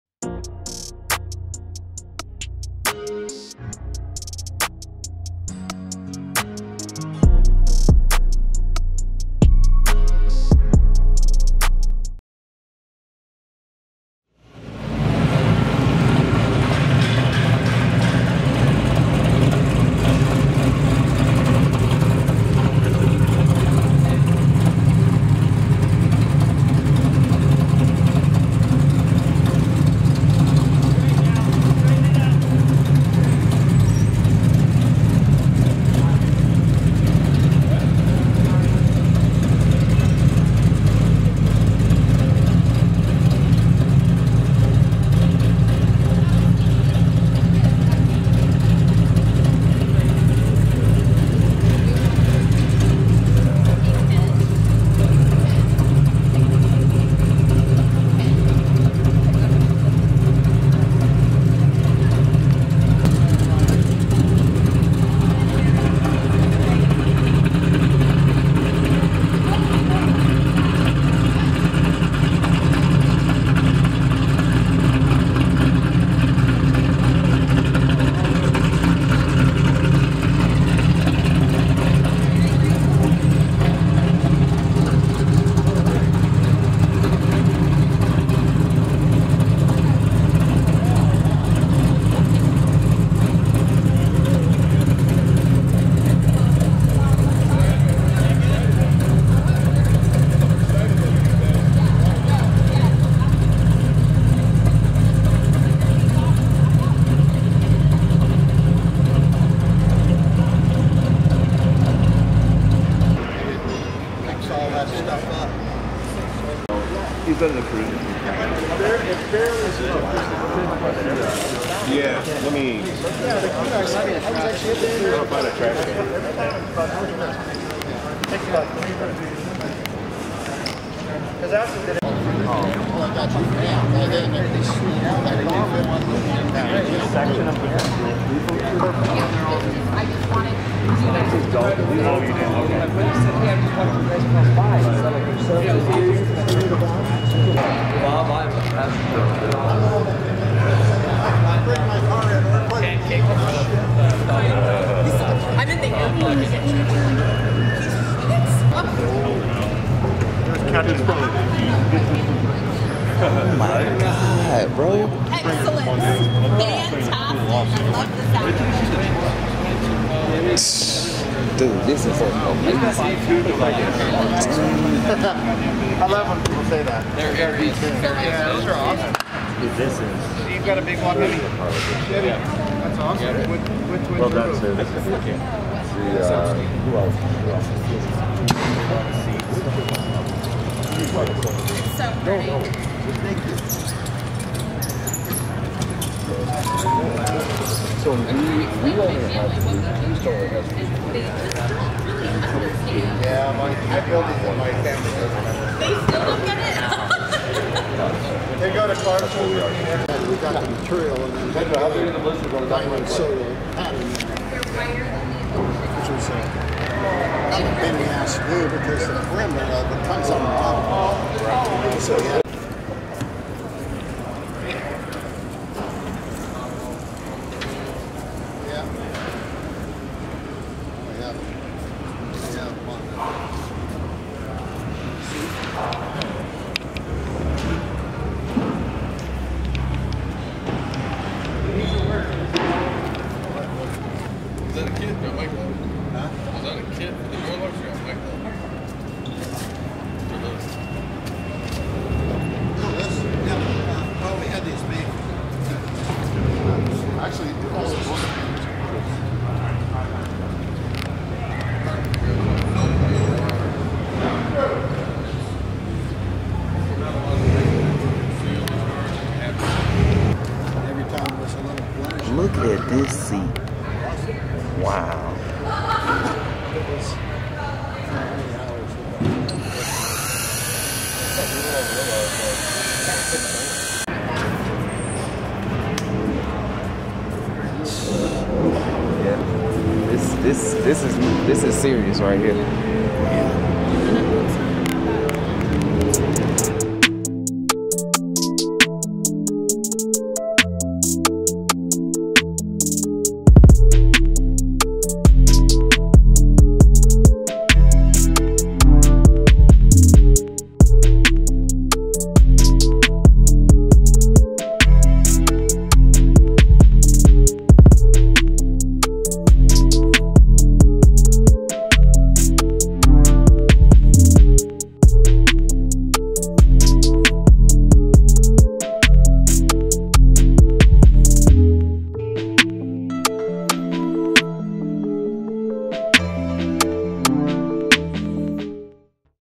WhipAddict: Took 10 Years To Build This Immaculate 55' Bel Air, Twin Turbo Big Block 540! Top Build
The Official Top Build of SEMA by the Battle Of The Builders Judges! The Blue 55' Bel Air 'Brute Force' is a work of art that took 10 years to complete with every piece touched! @bobmatranga and @kentmatranga Put together something special!